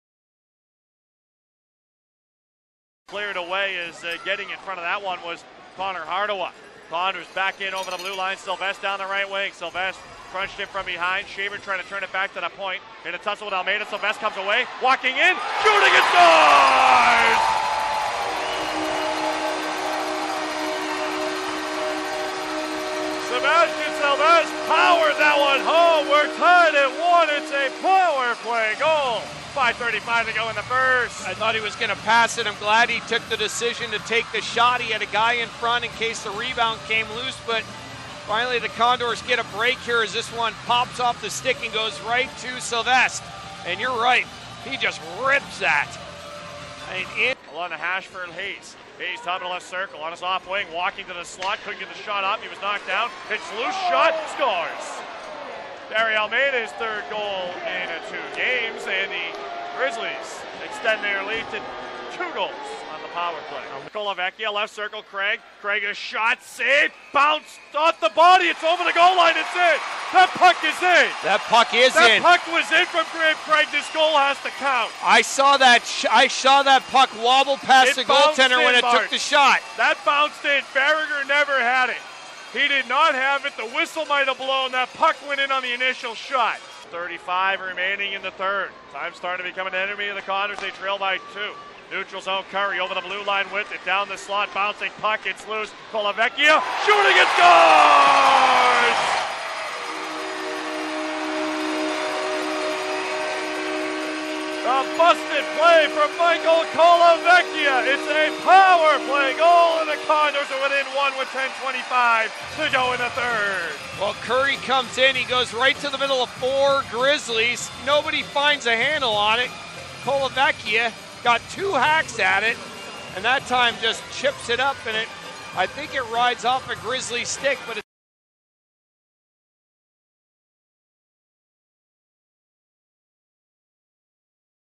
HIGHLIGHTS 1.17.15 | Utah 3, Condors 2
The Condors get goals from Sebastien Sylvestre and Michael Colavecchia.